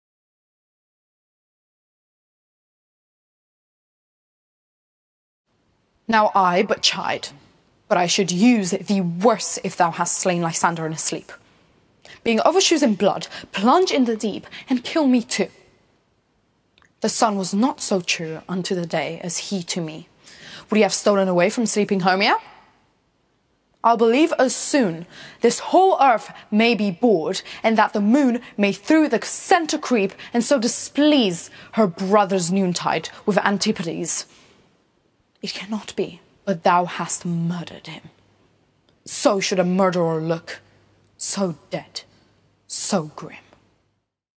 Shakespeare's Monologues || A Midsummer Night's Dream "Now I but chide; but I should use thee worse"
"Hermia" performed by Octavia Selena Alexandru.

Hermia (ACT III SCENE II) "Now I but chide; but I should use thee worse,"

"Now I but chide; but I should use thee worse,
If thou hast slain Lysander in his sleep,
Being o'er shoes in blood, plunge in the deep,
And kill me too.
The sun was not so true unto the day
As he to me: would he have stolen away
From sleeping Hermia? I'll believe as soon
This whole earth may be bored and that the moon
May through the centre creep and so displease
Her brother's noontide with Antipodes.
It cannot be but thou hast murder'd him;
So should a murderer look, so dead, so grim. "

Cinematography & Editing: The Insane Artist
1st AD and Script Supervisor: O. A. Alexandru

Hair & Make-up: The Insane Artist

My special thanks go to:
* The Acting/Dialect Coach, Mrs Julie Austin

* The Team and Dedicated Photography Tutors, Roland Pokrywka and Aga King

* Mum & Dad